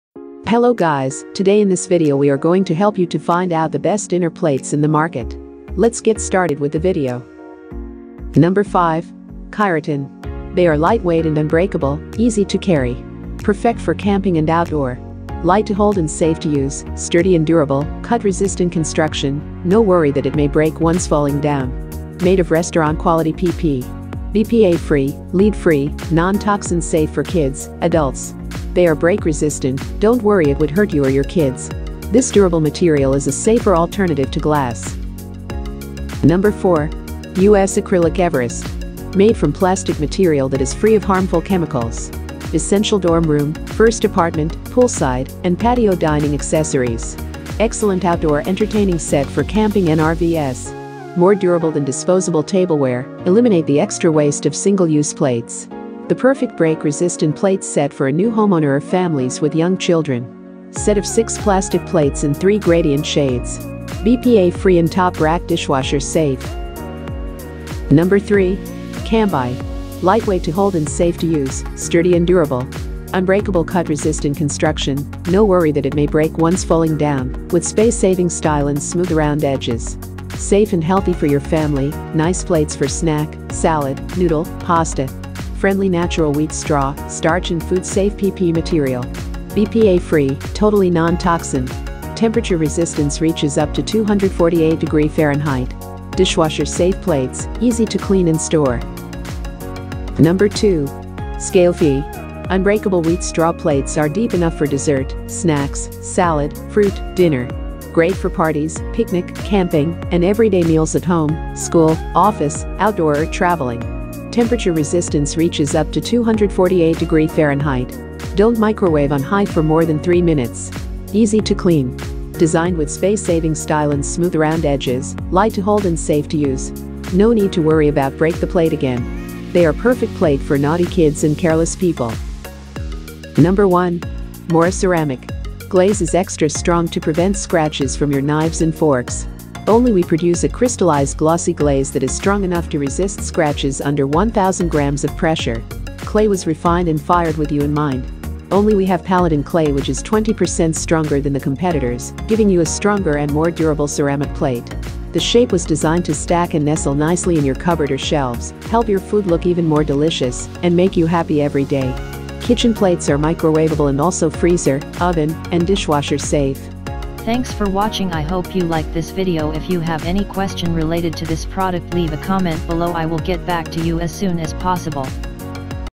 Dinner Plates : Top 5 Best Dinner Plates 2022
Do you need the best Dinner Plates. You welcome to watch out the top 5 Dinner Plates reviews videos. Tried to collect best items. If you want to know details about each product. Follow Below:

1. Mora Ceramic Dinner Plate

2. SGAOFIEE Dinner Plate

3. CAMBUY Wheat Straw Plate

4. US Acrylic Everest Dinner Plate

5. Kyraton Plastic Dinner Plate